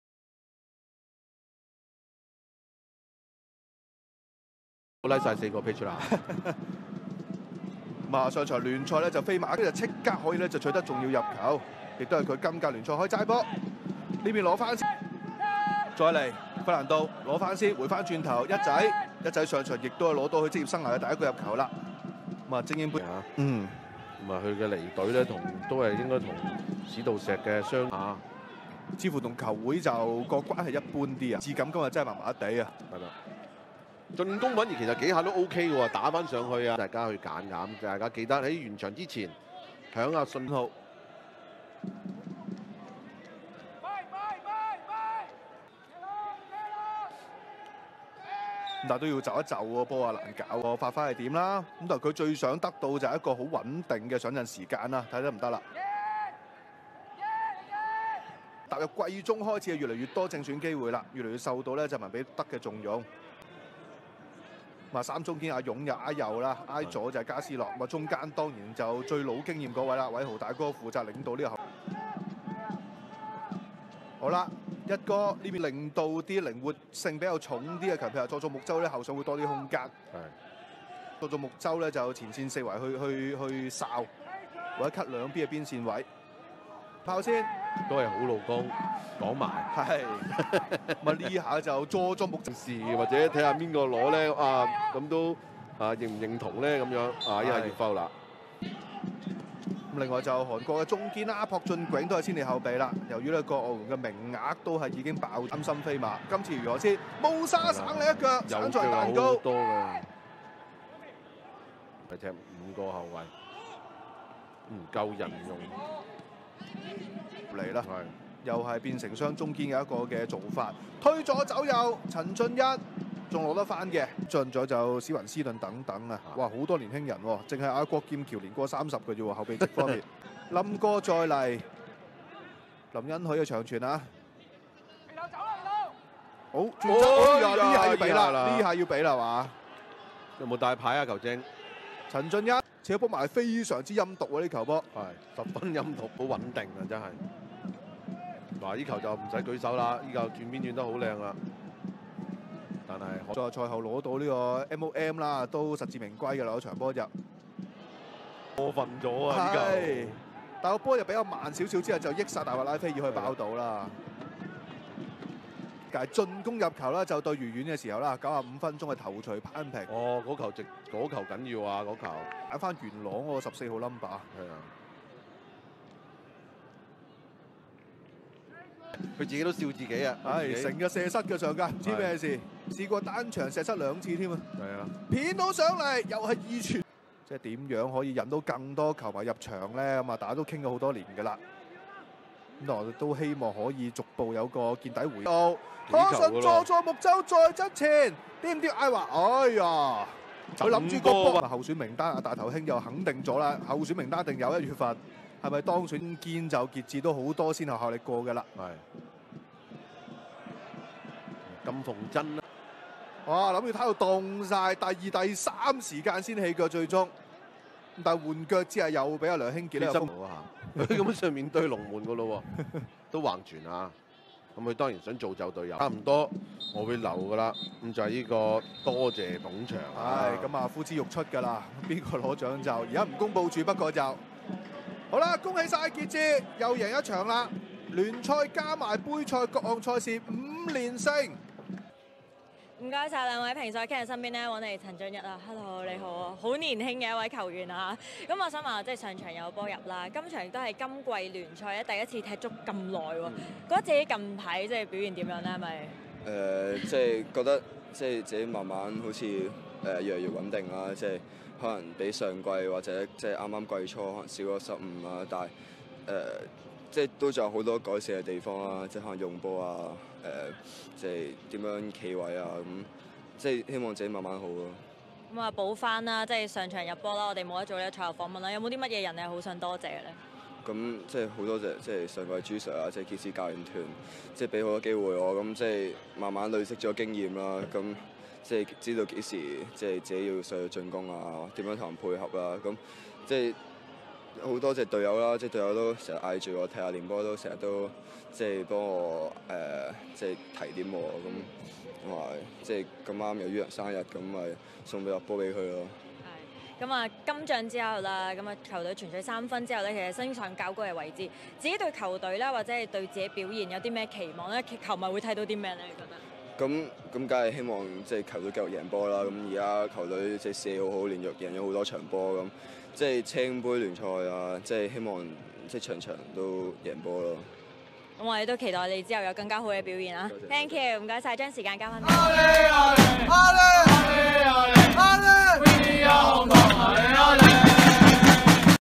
陳晉一 CHAN, Shinichi 港超聯 對飛馬 全場精華 Match Highlight 2020/01/16
This video contain Chan Shinichi against HK Pegasus  in Hong Kong Premier League 19/20
Name: 陳晉一 Chan Shinichi
Date of birth: Sep 5, 2002
Age: 17
Height: 1,83 m
Citizenship: Hong Kong
Position: Forward - Left Winger
Current club: Kitchee SC